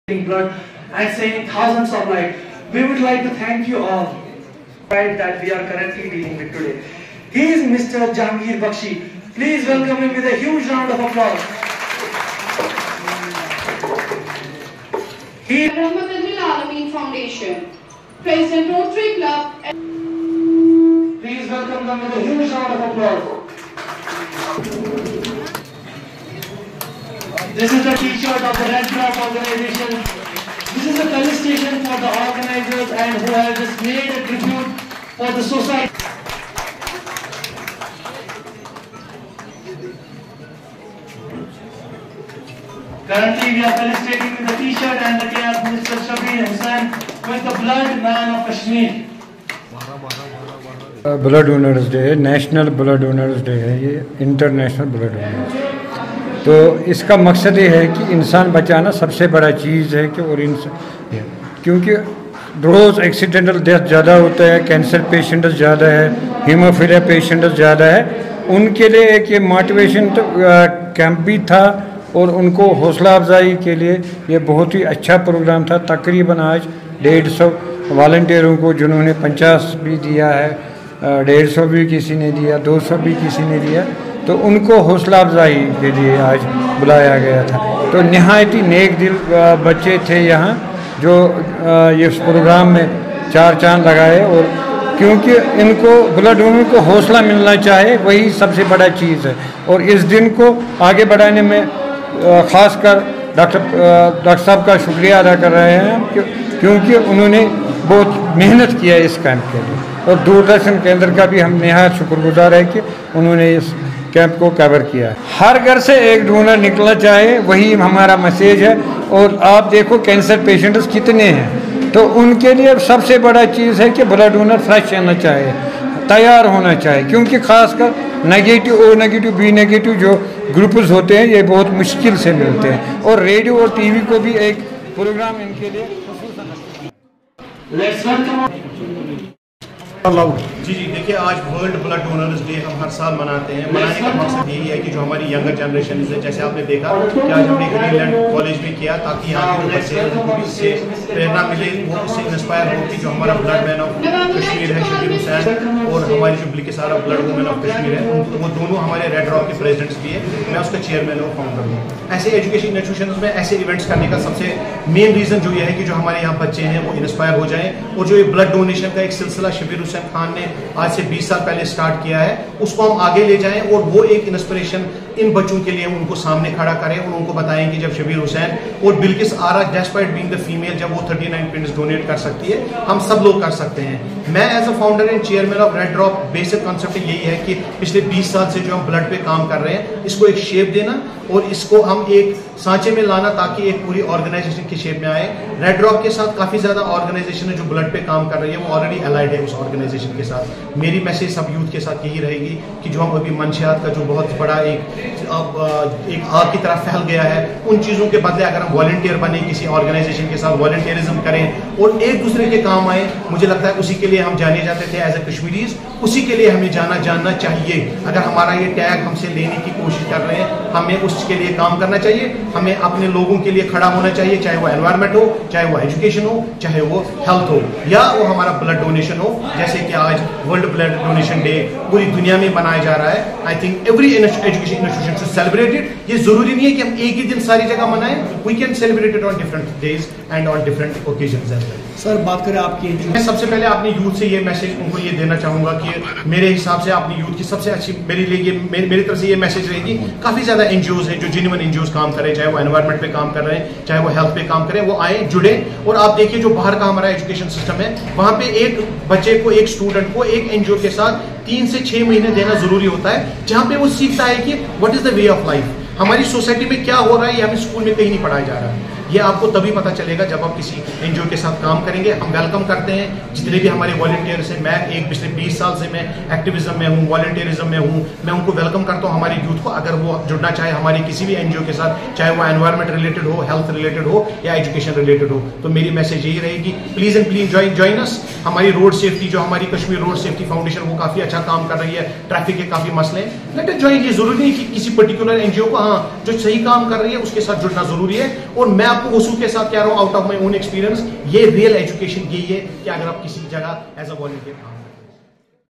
RED DROP - A Unit of Blood Donors Launched on World Blood Donors Day
Srinagar, Jammu & Kashmir - June 14, 2024 - On the occasion of World Blood Donors Day, a significant initiative was launched to honor and support blood donors in the region. RED DROP, a unit of Blood Donors, was officially inaugurated in a grand ceremony held at a prestigious venue in Srinagar. The event was graced by esteemed guests and dignitaries, including Chief Guest Javeed Ahmed Tenga (President of KCC&I) and Guest of Honour Mr. Jhangir Bakshi (Secretary of DLSA).

The inauguration ceremony was attended by prominent figures in the community, such as Astt Director Health Dr. Arjmand Bashir, Chairman of Baseera the Home (NGO) Sheikh Bashir, Chairman of Greenland Foundation Er. Suhail Jan, Chairman of Chote Tare Foundation Arjumand Maqddomi, and Chairman of Rehmat Sheikh Firdous Ali, Irfana Zargar (Pad women of Kashmir)President Rotary Club of Kashmir Sdr Manjeet singh Chairman KRSF Nasir Ali Khan,Sayyed Parvez Qalander Representatives from Trade Altaf Ahmed Dar and others Syed Ajaz Khashani Adv Syed Mujtaba Adv Asif Rashid ,Adv Asrar Ali , M amin Dar,Shahid Khan Fajr (Youngest Donor) Imran Zargar (Co ordinator ) Fajad Sultan Team DLSA ,RED DROP whole Team ,Principal ,Faculty members and students of Greenland Foundation and Volunteers from various organizations.Their presence underscored the importance of promoting blood donation and supporting those in need.

Dr. Touseef, Chairman and Founder of RED DROP, along with Shabir Hussain President RED DROP (known as the Blood Man of Kashmir) and Bilkees Ara President RED DROP President Women wing (known as the Blood Women of Kashmir), felicitated the blood donors for their selfless contributions to saving lives. Dr. Touseef Ahmed delivered a poignant address highlighting the critical importance of regular blood donations in ensuring the availability of life-saving blood for those in need.

The event was a celebration of compassion, generosity, and community spirit, as individuals and organizations came together to support a noble cause. RED DROP aims to continue its mission of promoting blood donation, raising awareness, and ensuring a stable and sufficient blood supply for hospitals and patients in Kashmir.